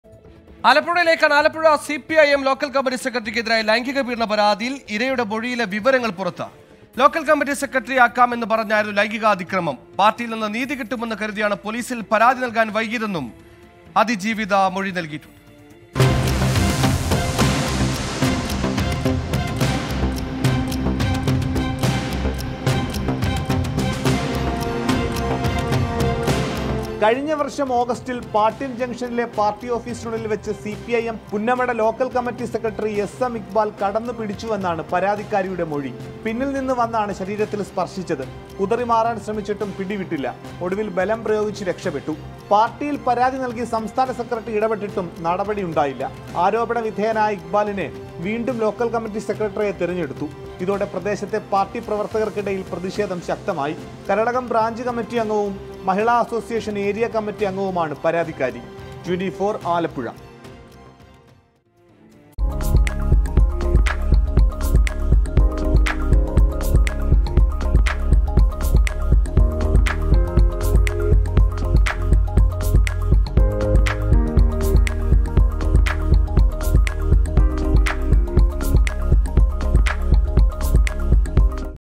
CPIM നേതാവിനെതിരായ ലൈംഗീക പീഡന പരാതി; ഇരയുടെ മൊഴിയിലെ വിവരങ്ങൾ പുറത്ത്
ആലപ്പുഴ സിപിഐഎം ലോക്കൽ കമ്മിറ്റി സെക്രട്ടറിക്ക് എതിരായ ലൈംഗീക പീഡന പരാതിയിൽ ഇരയുടെ മൊഴിയിലെ വിവരങ്ങൾ പുറത്ത്
Alappuzha | CPIM local committee secretary